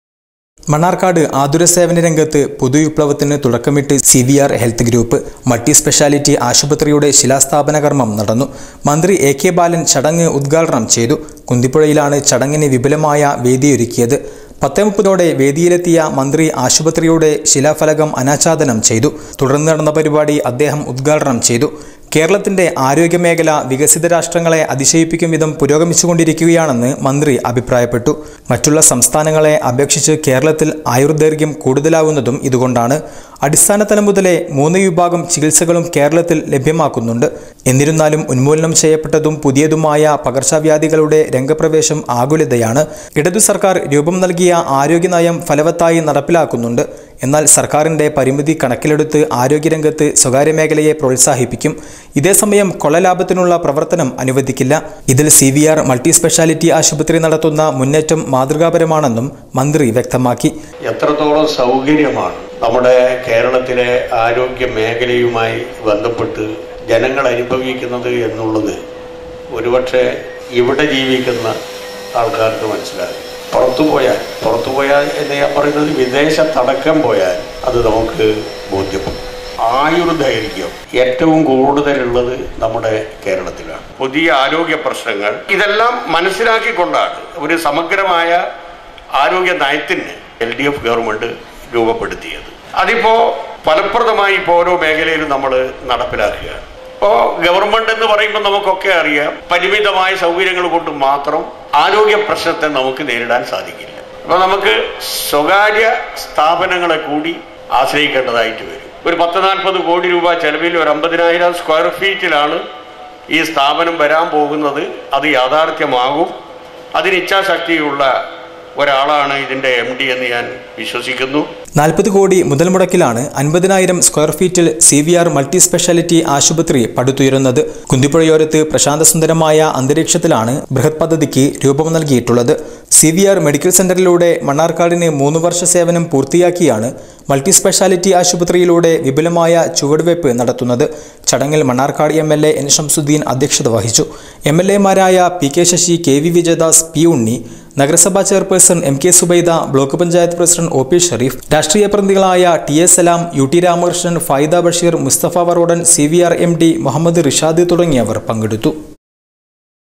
സി വി ആർ മൾട്ടി സ്പെഷ്യലിറ്റി ആശുപത്രിയുടെ ശിലാസ്ഥാപനം മന്ത്രി എ.കെ ബാലന്‍ നിര്‍വ്വഹിച്ചു.
സി വി ആർ മൾട്ടി സ്പെഷ്യലിറ്റി ആശുപത്രിയുടെ ശിലാസ്ഥാപനം മന്ത്രി എ.കെ ബാലന്‍ നിര്‍വ്വഹിച്ചു. ആരോഗ്യ രംഗത്തെ സി.വി.ആറിന്റെ മുന്നേറ്റം മാതൃകാപരമാണെന്ന് മന്ത്രി.